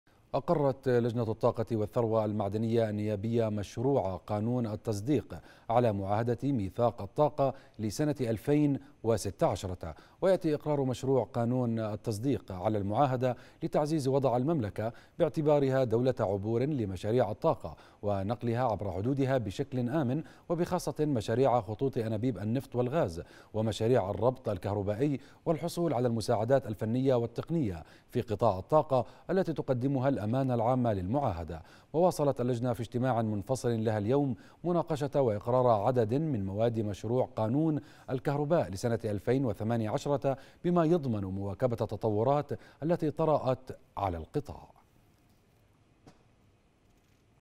"الطاقة النيابية" تقر مشروع قانون ميثاق الطاقة لسنة 2016 - (11-3-2018)
اقرت لجنةُ الطاقةِ والثروةِ المعدنيةِ النيابيةُ في اجتماعها اليوم مشروعَ قانونِ التصديق على معاهدةِ ميثاق الطاقة لسنةِ 2016.